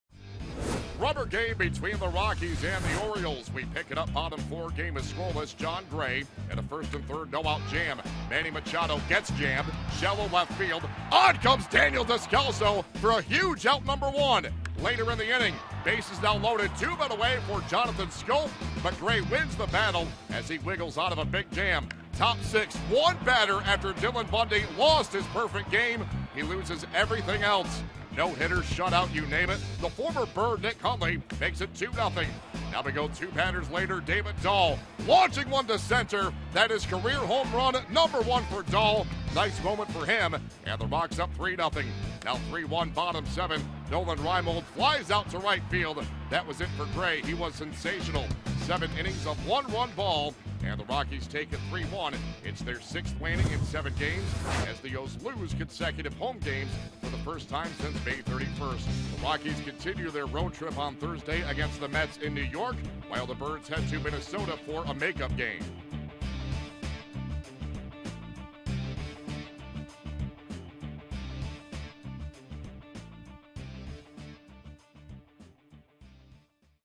7/27/16: Hundley, Dahl propel Rockies past Orioles
Daily Recap: Nick Hundley and David Dahl both homered and Jon Gray tossed seven innings of one-run ball in a 3-1 win vs. the Orioles